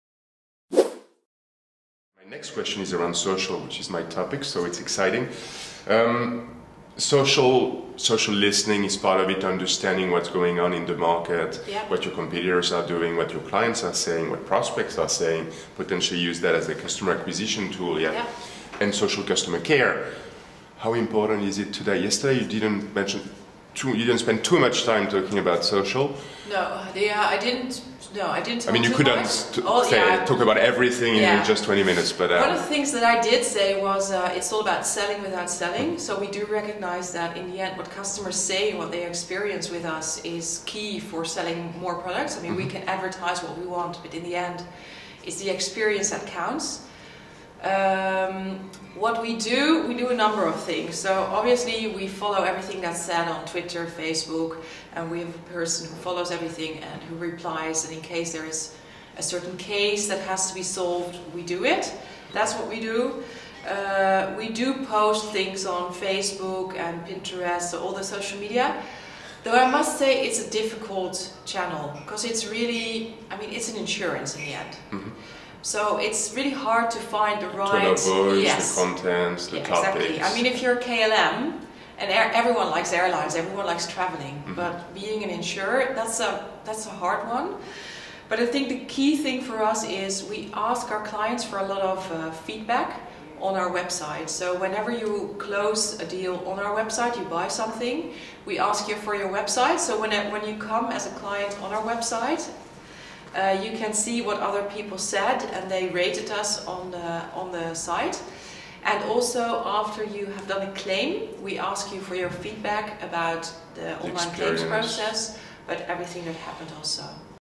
InShared Social Media Insurance CONTENT STRATEGY
This is a short extract of the 20mn video interview I recorded with Irene Van Den Brink - Director Business Development at InShared, during the EFMA Distribution Summit 2015 which took place in March 2015 in Barcelona.

Irene and I were both contributing to the Digital Banking and Social Media stream.

Your comments and your questions are more than welcome.